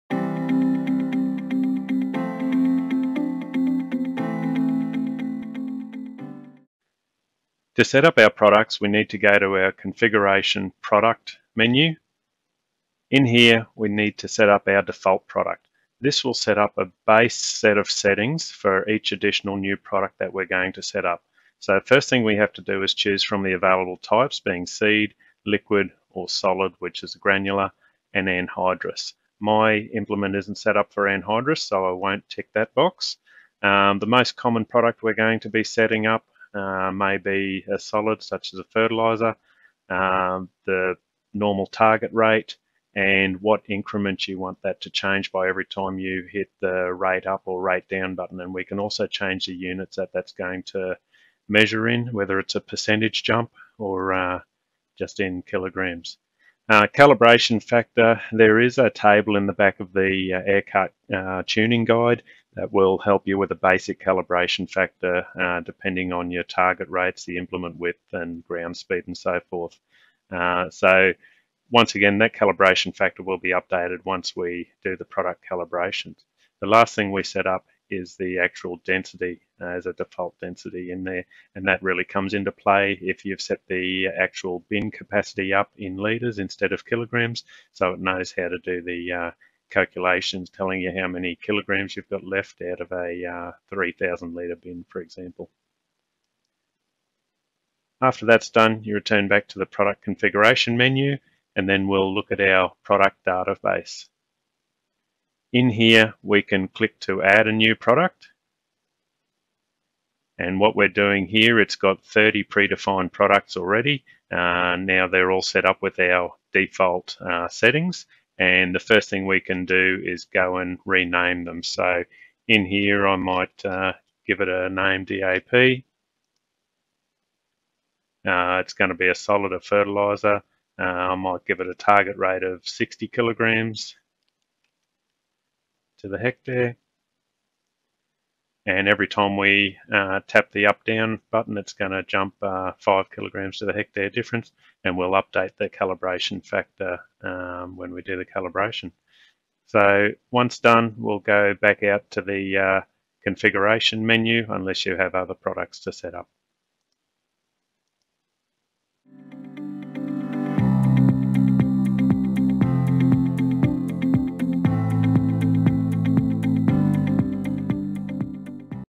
Midi 3 0 ISO ECU Seeder/Strip Till Applicator Product Setup
In this brief tutorial video you will be guided through how to create products so that you can assign them to a bin/tank on a seeder/strip till applicator that is being controlled by a Midi 3.0 ISO ECU.

or your local Case IH Dealer.